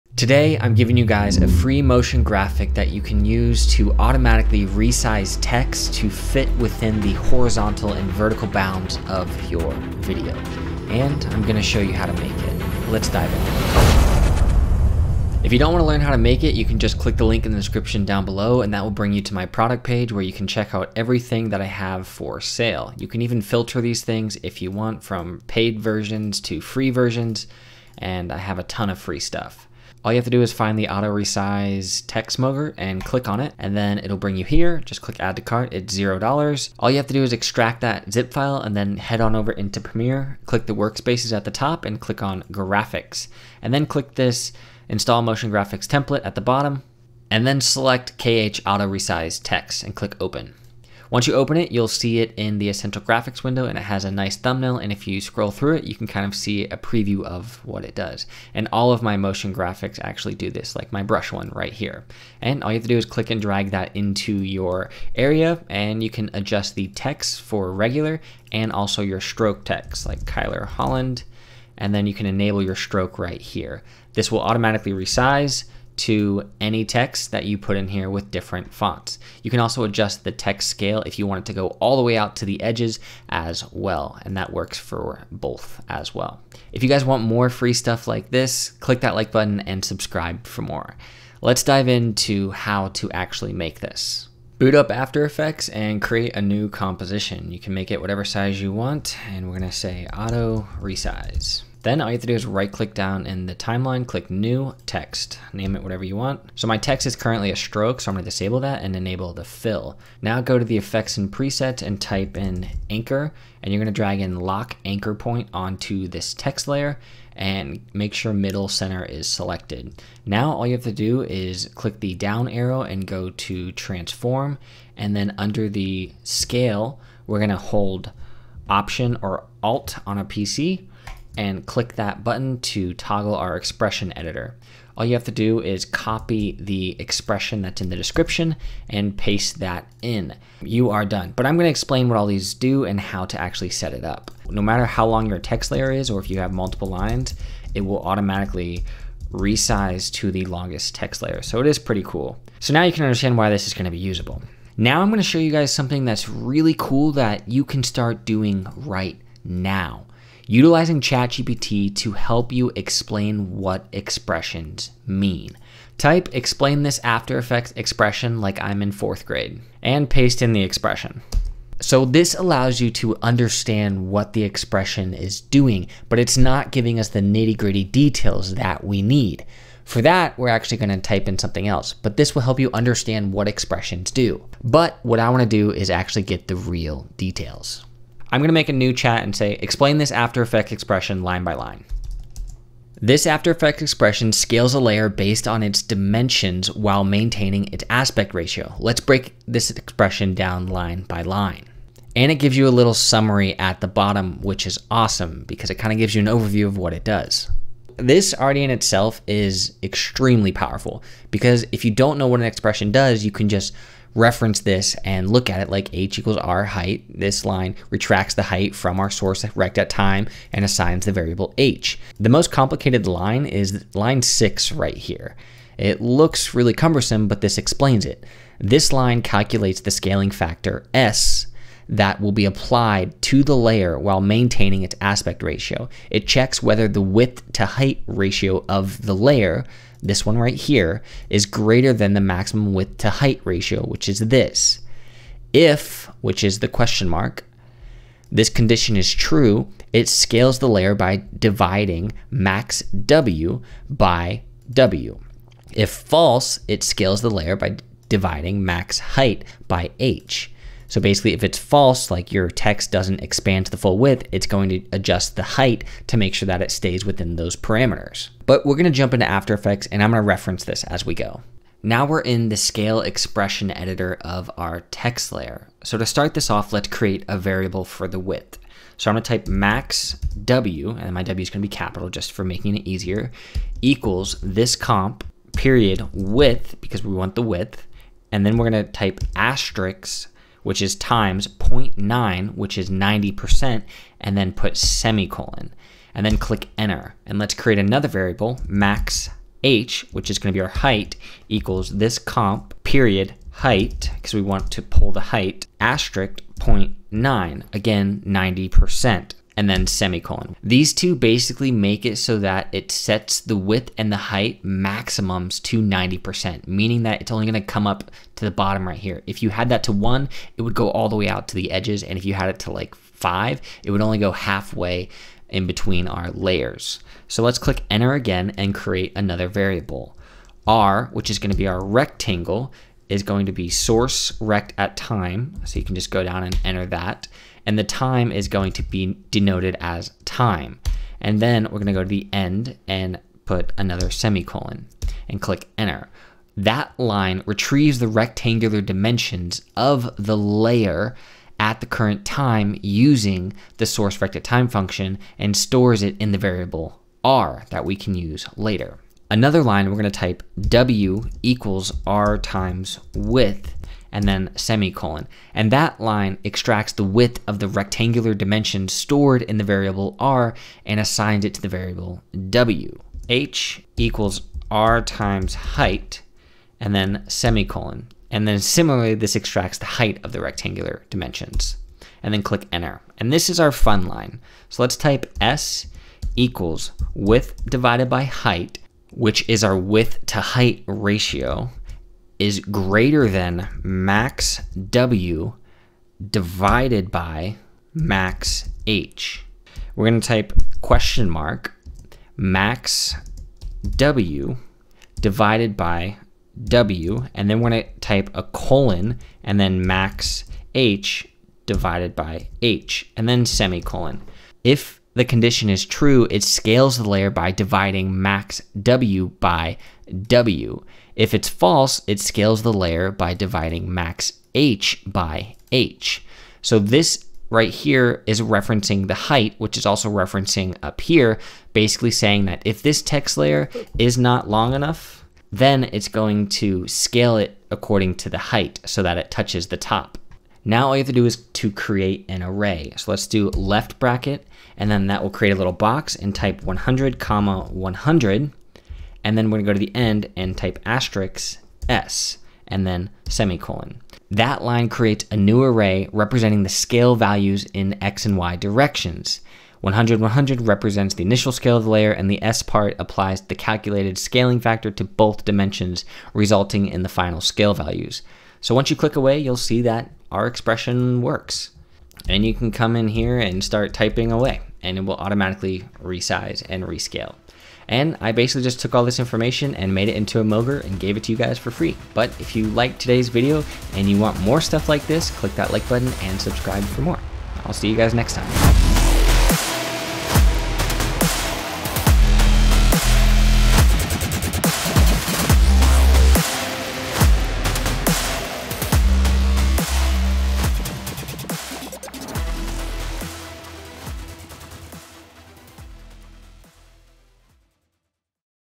FREE Auto Resize Text Motion Graphic + Tutorial | Premiere Pro + After Effects
How to make an Auto Resize Text Motion Graphic + use ChatGPT to help! 🔥

Refer to the Pinned Comment for the full Expression.

In this Premiere Pro tutorial, learn how to create an auto-resize text motion graphic that dynamically adjusts to fit within your video's dimensions. Easily enhance your video's visual appeal with this versatile technique. premierepro after effects

👀 CHAPTERS 👀
00:00 | Introduction to the Motion Graphic and Free Offer
00:15 | Get the Free Motion Graphic
00:36 | Installing the Motion Graphic Template in Premiere
00:50 | Customizing the Motion Graphic Template
01:26 | Breakdown of After Effects Expression
02:34 | Using ChatGPT for Expression Explanation
03:04 | Understanding Expression Line by Line
04:27 | Applying Expression in After Effects
06:55 | Explanation of Expression
07:45 | Conclusion